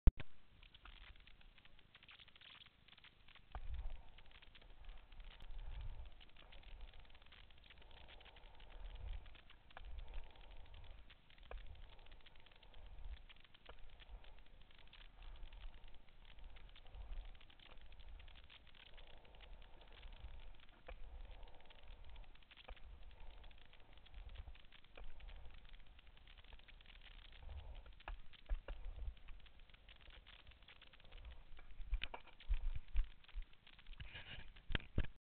Patinando por el nuevo Skatepark de Gijón
Video grabado por el rider Rubén Fernández con la Go Pro Surf Hero el 21/02/2010. Promueve